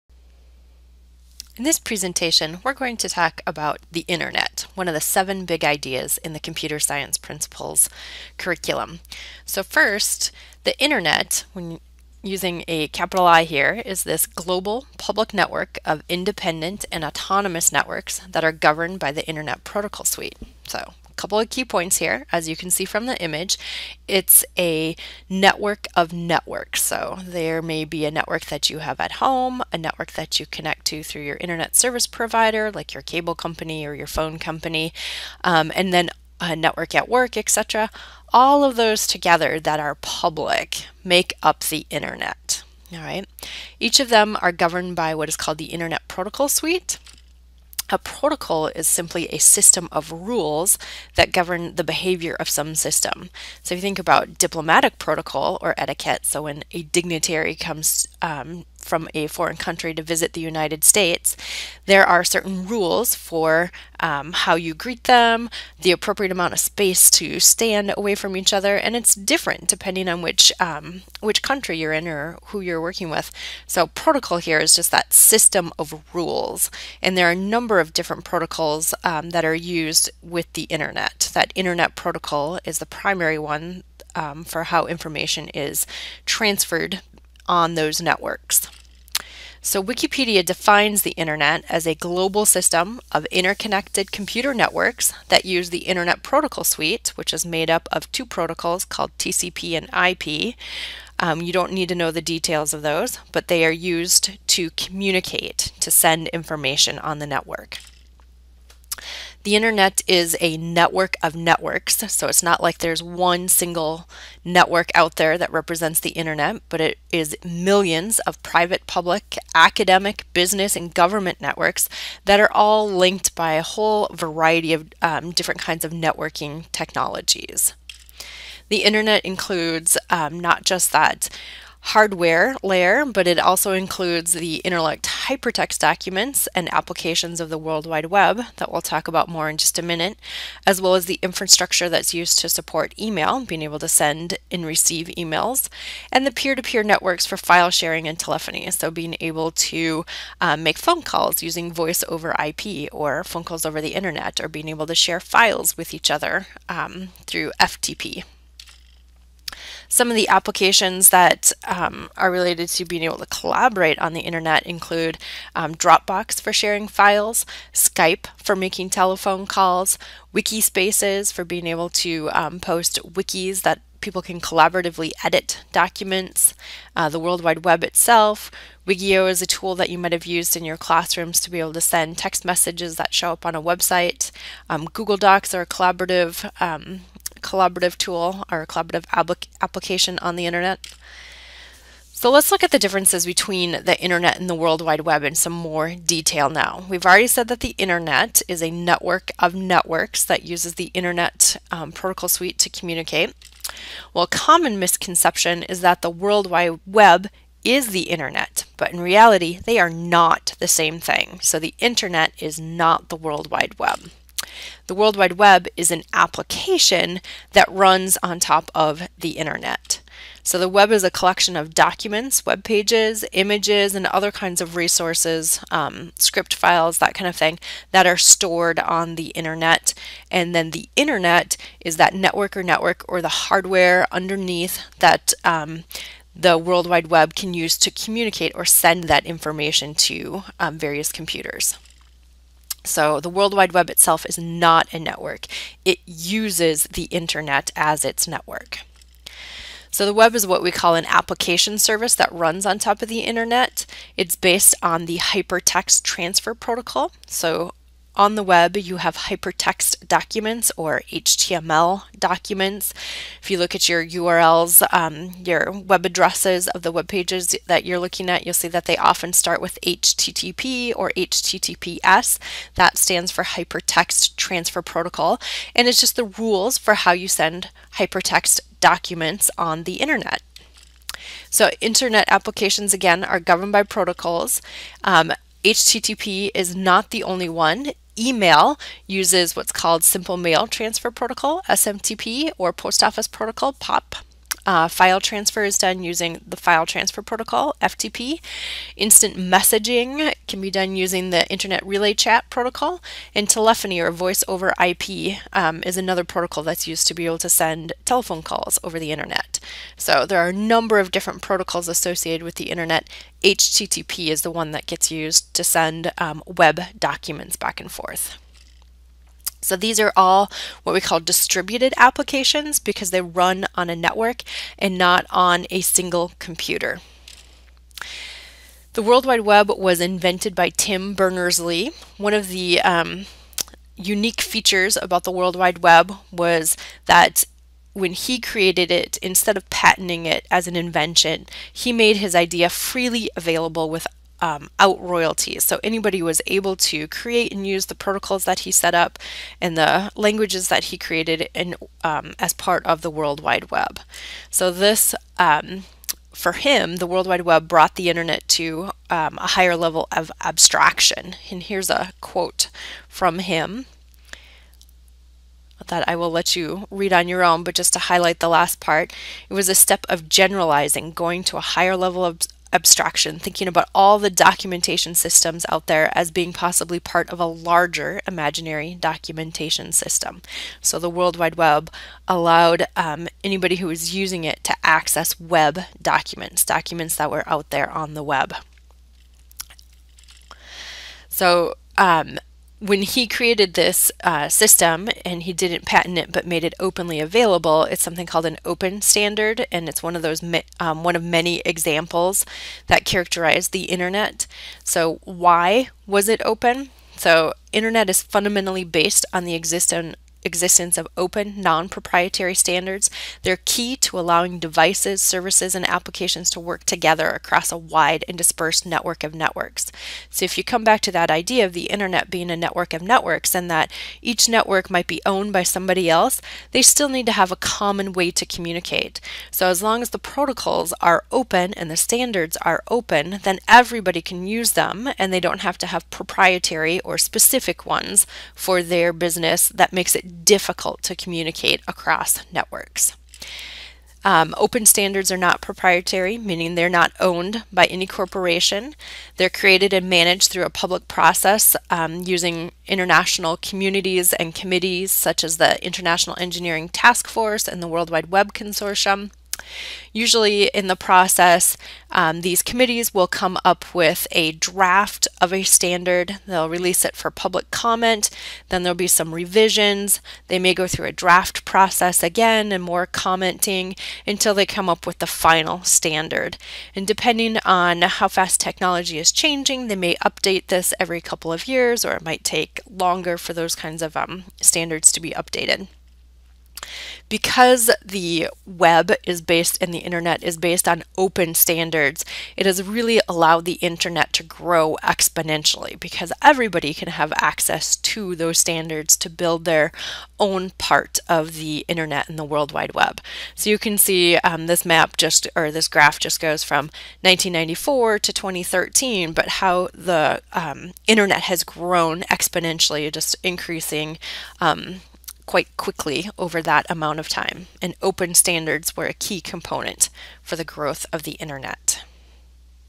What is the Internet?
Mobile CSP: What is the Internet presentation covers the basics of the Internet terminology and how it's related to the WWW